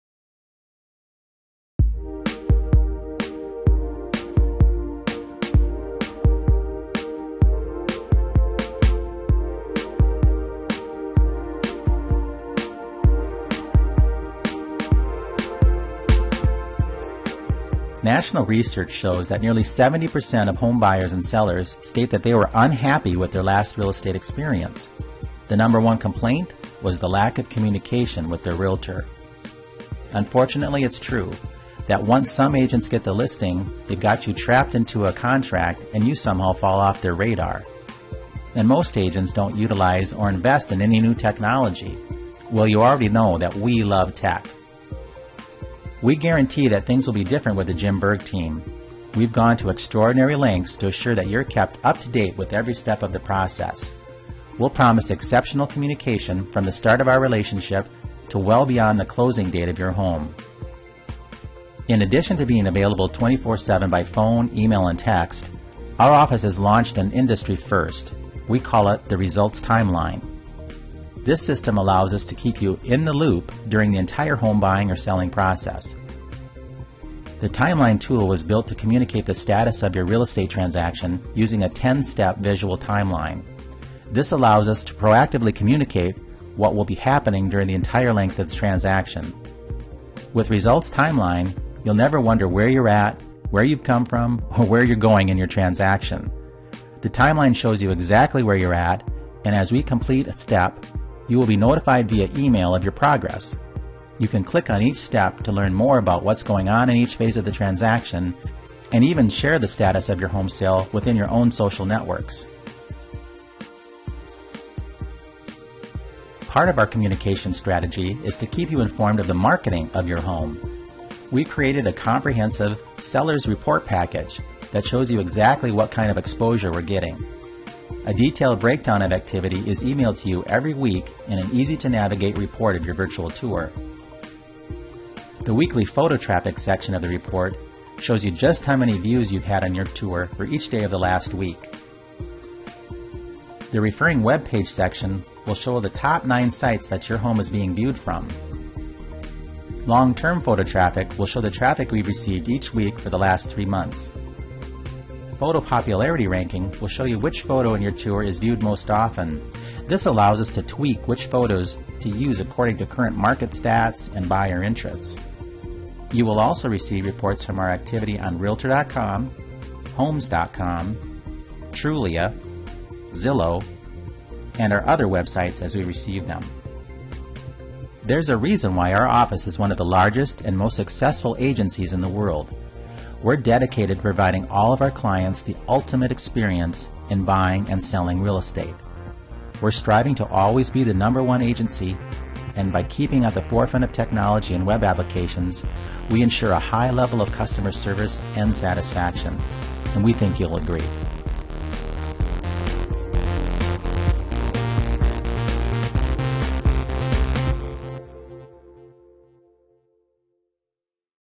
How We Work With Home Sellers
Jim discusses his background and approach to working with new clients.  Home selling shouldn't be stressful... and your visit with a Realtor shouldn't be either:)